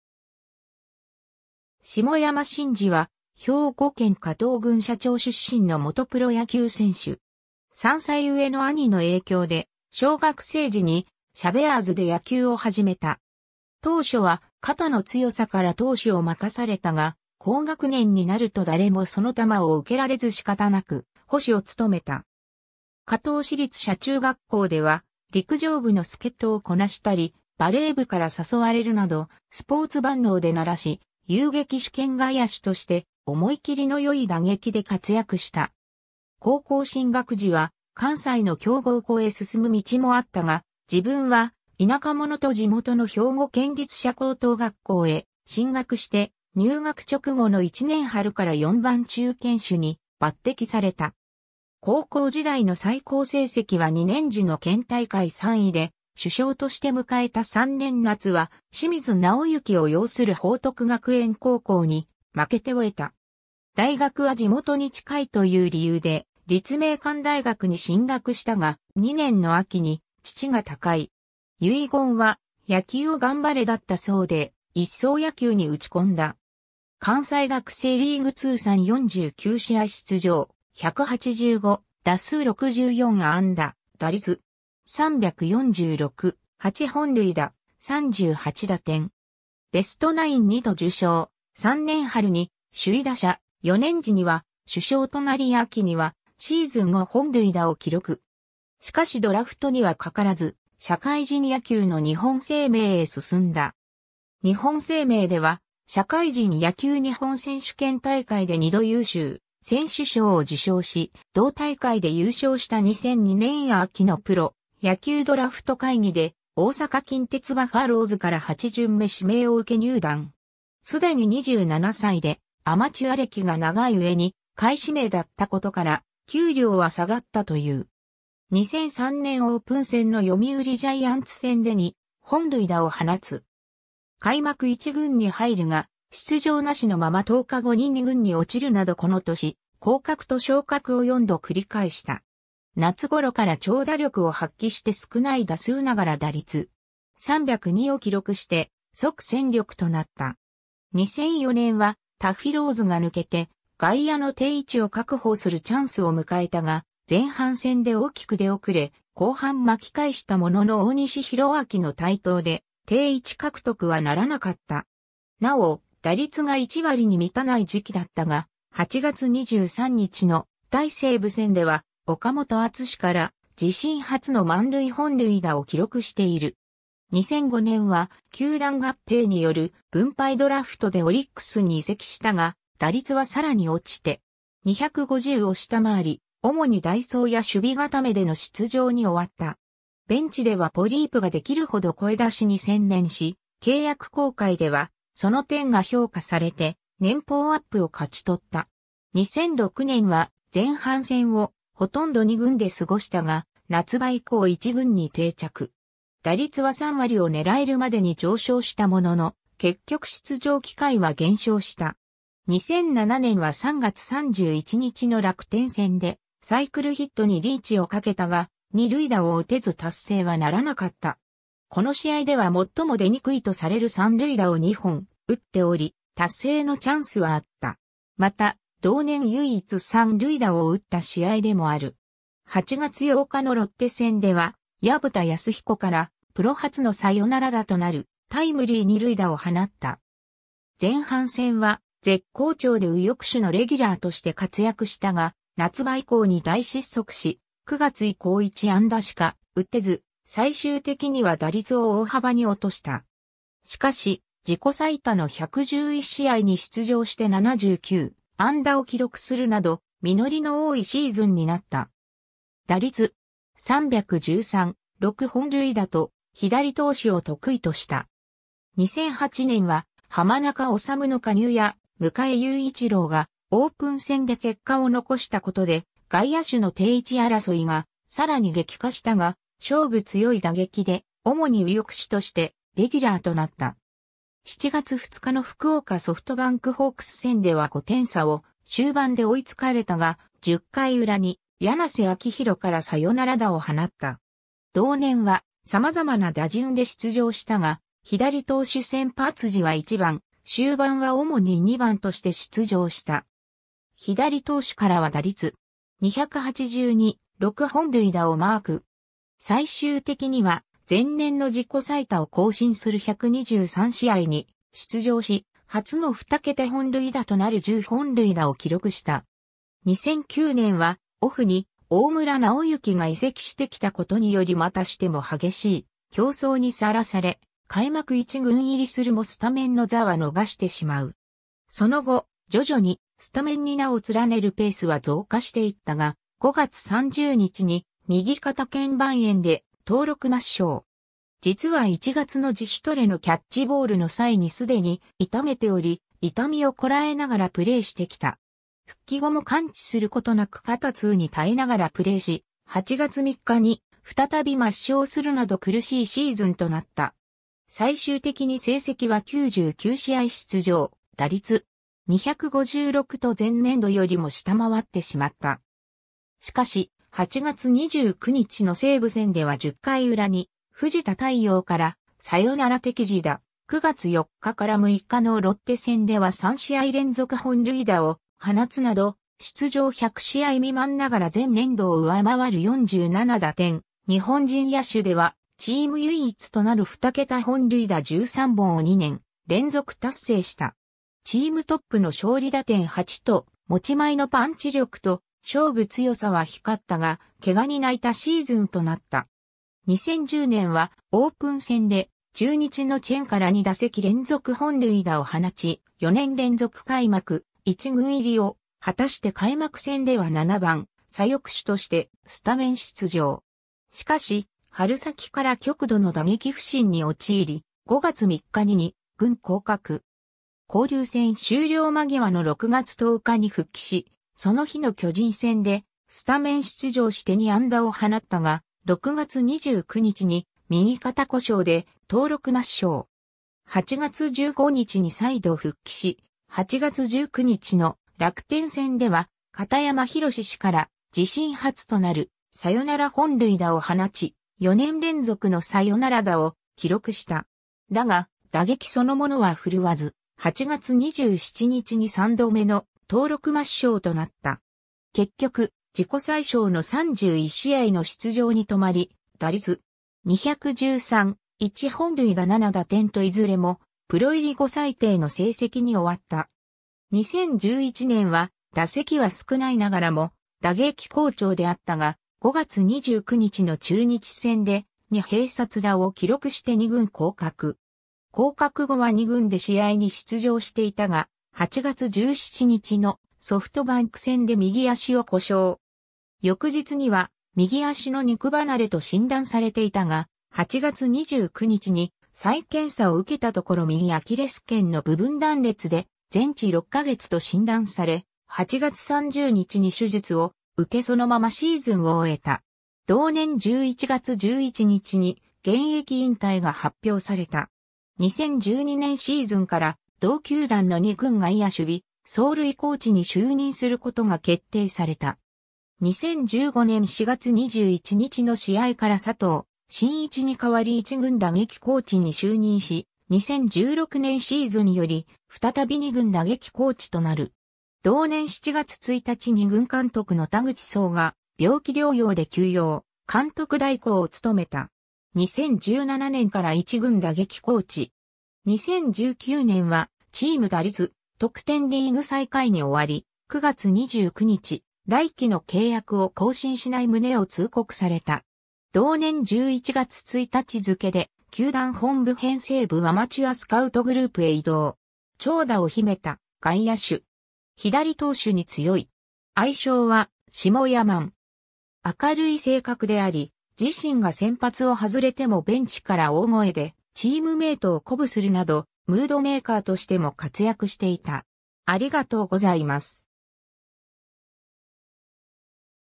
下山真二
下山 真二（しもやま しんじ、1975年12月1日 - ）は、兵庫県加東郡社町（現：加東市）出身の元プロ野球選手（外野手、右投右打）。
 3歳上の兄の影響で小学生時に「社ベアーズ」で野球を始めた。
当初は肩の強さから投手を任されたが、高学年になると誰もその球を受けられず仕方なく捕手を務めた。
加東市立社中学校では陸上部の助っ人をこなしたりバレー部から誘われるなどスポーツ万能で鳴らし、遊撃手兼外野手として思い切りの良い打撃で活躍した。
高校進学時は関西の強豪校へ進む道もあったが、「自分は田舎者」と地元の兵庫県立社高等学校へ進学して入学直後の1年春から4番中堅手に抜擢された。
高校時代の最高成績は2年時の県大会3位で、主将として迎えた3年夏は清水直行を擁する報徳学園高校に負けて終えた。
 大学は地元に近いという理由で立命館大学に進学したが、2年の秋に父が他界。
遺言は「野球を頑張れ」だったそうで、一層野球に打ち込んだ。
関西学生リーグ通算49試合出場、185打数64安打、打率.346、8本塁打、38打点。
ベストナイン2度受賞、3年春に首位打者、4年時には主将となり秋にはシーズン5本塁打を記録。
しかしドラフトにはかからず、社会人野球の日本生命へ進んだ。
 日本生命では社会人野球日本選手権大会で2度優秀選手賞を受賞し、同大会で優勝した2002年秋のプロ野球ドラフト会議で大阪近鉄バファローズから8巡目指名を受け入団。
既に27歳でアマチュア歴が長い上に下位指名だったことから、給料は下がったという。
 2003年オープン戦の読売ジャイアンツ戦で2本塁打を放つ。
開幕一軍に入るが出場無しのまま10日後に二軍に落ちるなどこの年降格と昇格を4度繰り返した。
夏頃から長打力を発揮して少ない打数ながら打率.302を記録して即戦力となった。
 2004年はタフィ・ローズが抜けて外野の定位置を確保するチャンスを迎えたが前半戦で大きく出遅れ、後半巻き返したものの大西宏明の台頭で定位置獲得はならなかった。
なお、打率が1割に満たない時期だったが、8月23日の対西武戦では岡本篤志から自身初の満塁本塁打を記録している。
 2005年は球団合併による分配ドラフトでオリックスに移籍したが、打率はさらに落ちて.250を下回り、主に代走や守備固めでの出場に終わった。
ベンチではポリープができるほど声出しに専念し、契約更改ではその点が評価されて年俸アップを勝ち取った。
 2006年は、前半戦を殆ど二軍で過ごしたが、夏場以降一軍に定着。
打率は3割を狙えるまでに上昇したものの、結局出場機会は減少した。
 2007年は3月31日の楽天戦でサイクルヒットにリーチをかけたが、二塁打を打てず達成はならなかった。
この試合では最も出にくいとされる三塁打を2本打っており、達成のチャンスはあった。
また、同年唯一三塁打を打った試合でもある。
8月8日のロッテ戦では、薮田安彦からプロ初のサヨナラ打となるタイムリー二塁打を放った。
前半戦は絶好調で右翼手のレギュラーとして活躍したが夏場以降に大失速し、9月以降1安打しか打てず最終的には打率を大幅に落とした。
しかし自己最多の111試合に出場して79安打を記録するなど、実りの多いシーズンになった。
打率.313、6本塁打と左投手を得意とした。
 2008年は濱中治の加入や、迎祐一郎がオープン戦で結果を残した事で外野手の定位置争いが更に激化したが、勝負強い打撃で主に右翼手としてレギュラーとなった。
7月2日の福岡ソフトバンクホークス戦では5点差を終盤で追いつかれたが、10回裏に柳瀬明宏からサヨナラ打を放った。
同年は様々な打順で出場したが、左投手先発時は1番、終盤は主に2番として出場した。
左投手からは打率.282、6本塁打をマーク。
最終的には前年の自己最多を更新する123試合に出場し、初の2桁本塁打となる10本塁打を記録した。
 2009年はオフに大村直之が移籍してきたことによりまたしても激しい競争に晒され、開幕1軍入りするもスタメンの座は逃してしまう。
その後、徐々にスタメンに名を連ねるペースは増加していったが、5月30日に右肩腱板炎で登録抹消。
実は1月の自主トレのキャッチボールの際にすでに痛めており、痛みをこら...